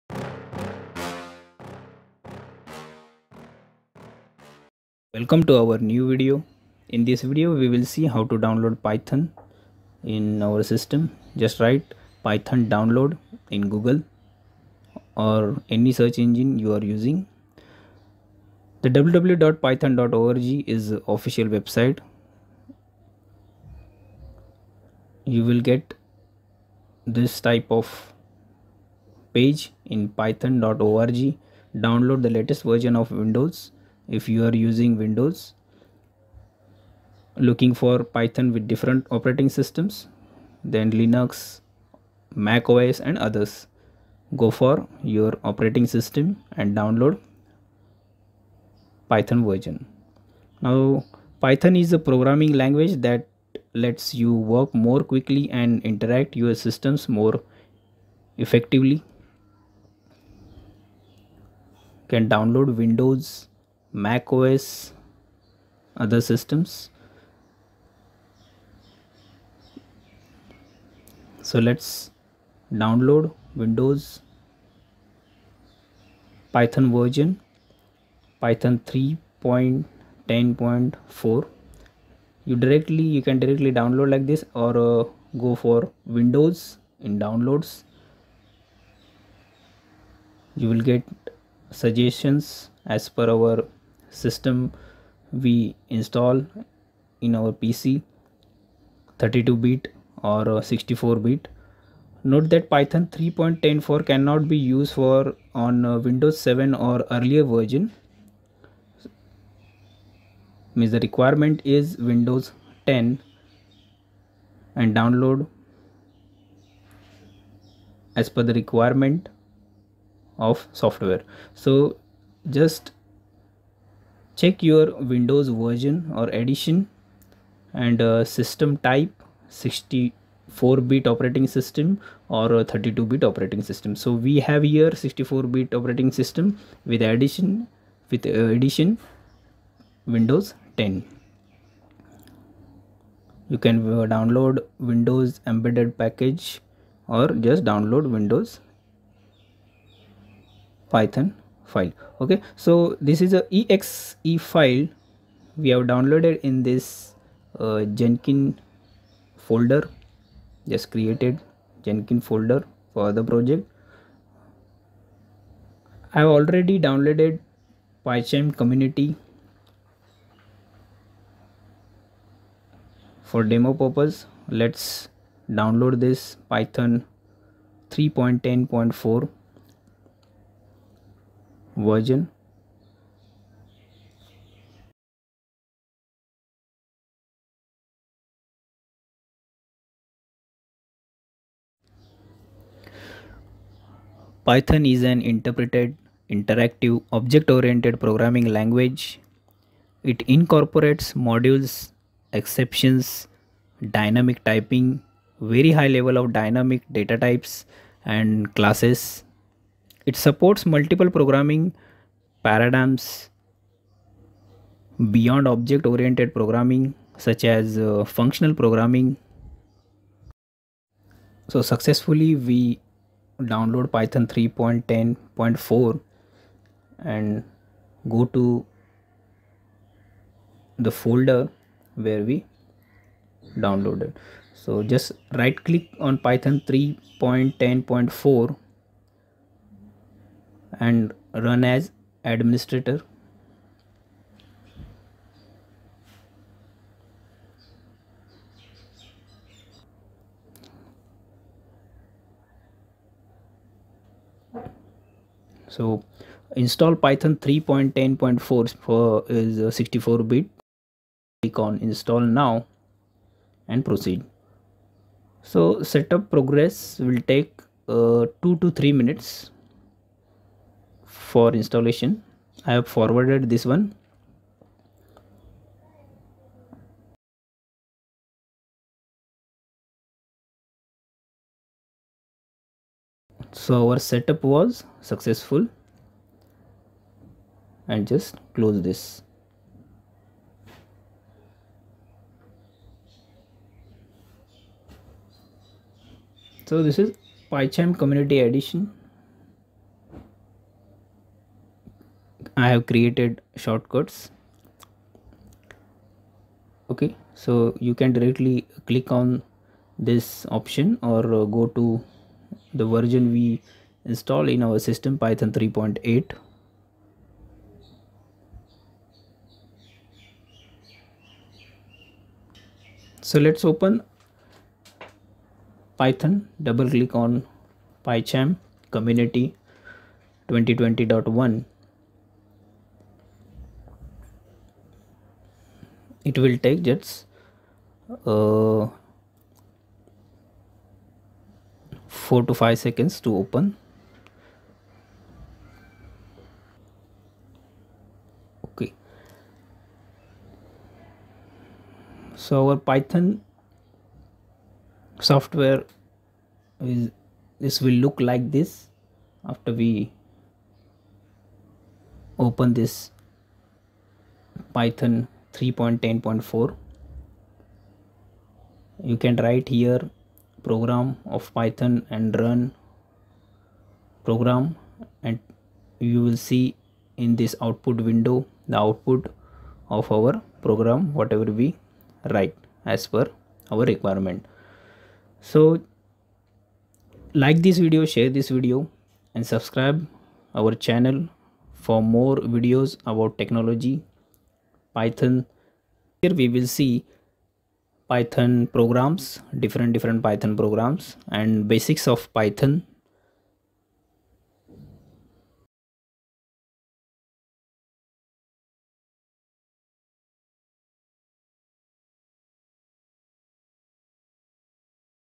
How to install Python 3.10.4 on Windows 10
learn to download and install Python 3.10.0 on Windows 10.